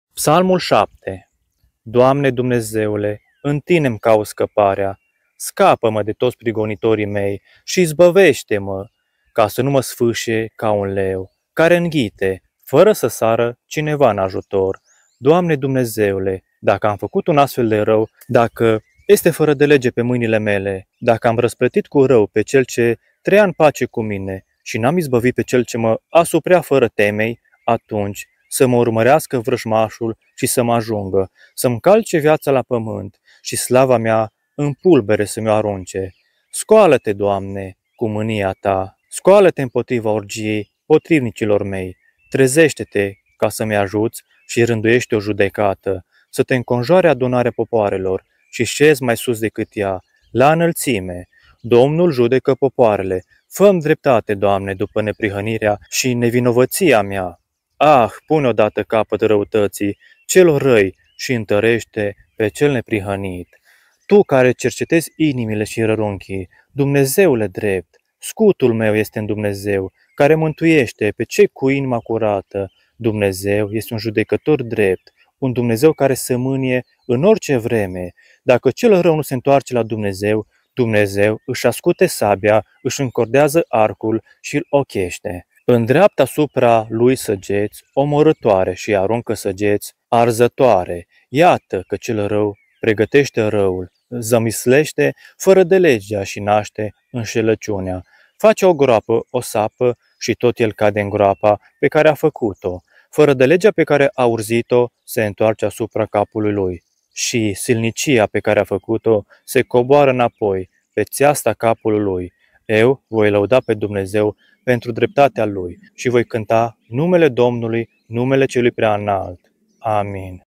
Dacă cel rău nu se întoarce la Dumnezeu, Dumnezeu Îşi ascute sabia-PSALMUL 7
Capitolul 7
1 Doamne Dumnezeule, în Tine îmi caut scăparea! Scapă-mă de toţi prigonitorii mei şi izbăveşte-mă,
2 ca să nu mă sfâşie ca un leu care înghite, fără să sară cineva în ajutor.
3 Doamne Dumnezeule, dacă am făcut un astfel de rău, dacă este fărădelege pe mâinile mele,
4 dacă am răsplătit cu rău pe cel ce trăia în pace cu mine, şi n-am izbăvit pe cel ce mă asuprea fără temei,
5 atunci: să mă urmărească vrăjmaşul şi să m-ajungă, să-mi calce viaţa la pământ, şi slava mea în pulbere să mi-o arunce! (Oprire)
6 Scoală-Te, Doamne, cu mânia Ta! Scoală-Te împotriva urgiei potrivnicilor mei, trezeşte-Te ca să-mi ajuţi şi rânduieşte o judecată!
7 Să Te înconjoare adunarea popoarelor, şi şezi mai sus decât ea, la înălţime!
8 Domnul judecă popoarele: fă-mi dreptate, Doamne, după neprihănirea şi nevinovăţia mea!
9 Ah! pune odată capăt răutăţii celor răi şi întăreşte pe cel neprihănit, Tu, care cercetezi inimile şi rărunchii, Dumnezeule drept!
10 Scutul meu este în Dumnezeu, care mântuieşte pe cei cu inima curată.
11 Dumnezeu este un judecător drept, un Dumnezeu care Se mânie în orice vreme.
12 Dacă cel rău nu se întoarce la Dumnezeu, Dumnezeu Îşi ascute sabia, Îşi încordează arcul şi-l ocheşte,
13 îndreaptă asupra lui săgeţi omorâtoare şi-i aruncă săgeţi arzătoare!
14 Iată că cel rău pregăteşte răul, zămisleşte fărădelegea şi naşte înşelăciunea:
15 face o groapă, o sapă, şi tot el cade în groapa pe care a făcut-o.
16 Fărădelegea pe care a urzit-o se întoarce asupra capului lui, şi silnicia pe care a făcut-o se coboară înapoi pe ţeasta capului lui.
17 Eu voi lăuda pe Domnul pentru dreptatea Lui şi voi cânta Numele Domnului, Numele Celui Preaînalt.